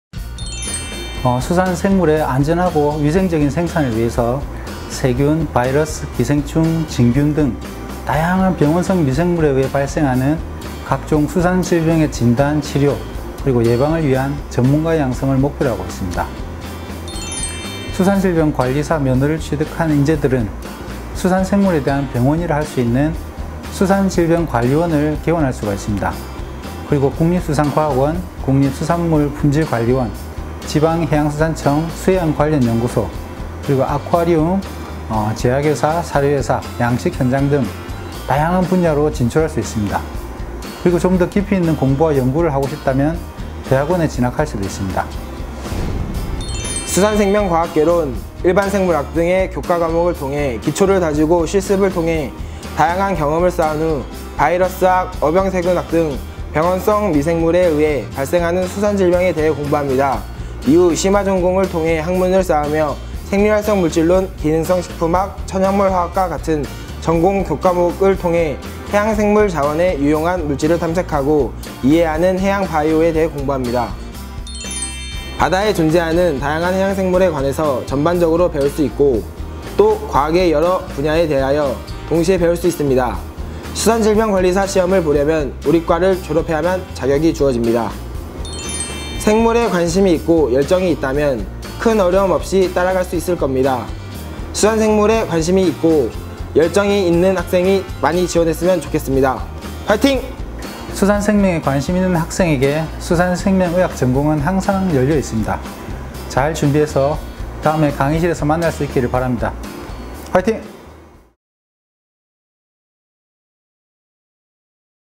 [하이틴TV] 제주대학교-수산생명의학전공
아시아의 명문, 세계의 중심,
글로벌 중심 대학으로 거듭나는 제주대학교
수산생명의학전공을 소개합니다!